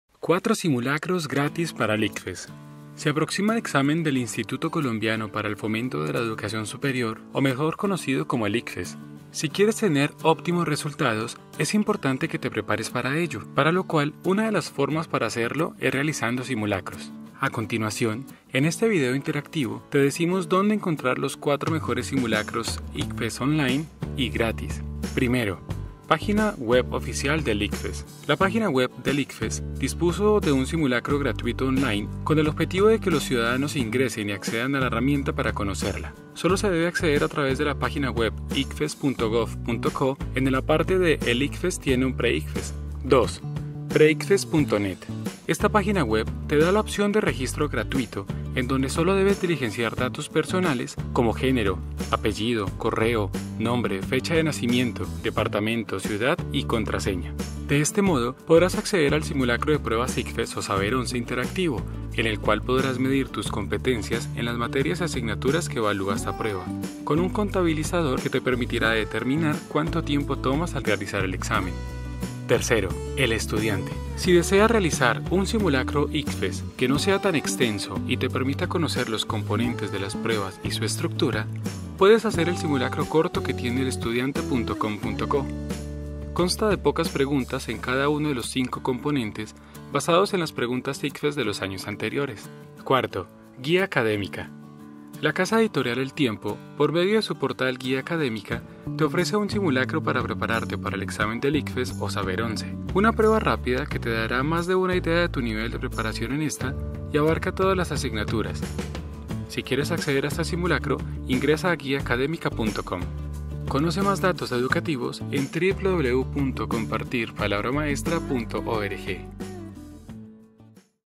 4 paginas GRATIS para hacer SIMULACROS del ICFES
Si próximamente presentas el examen del Instituto Colombiano para el Fomento de la Educación Superior (Icfes) y quieres obtener óptimos resultados es importante que te prepares idóneamente para ello, para lo cual una de las formas en que puedes prepararte es, realizando simulacros de pruebas Saber online.

En este video interactivo te contamos dónde encontrar los 4 mejores simulacros Icfes online gratis, para que puedas utilizar como guía a la hora de prepararte y hacerte una idea de cómo será esta evaluación, de qué está compuesta, cuánto tiempo dispones para realizarla y qué temas abordará, entre otros aspectos.

1. Página web oficial del Icfes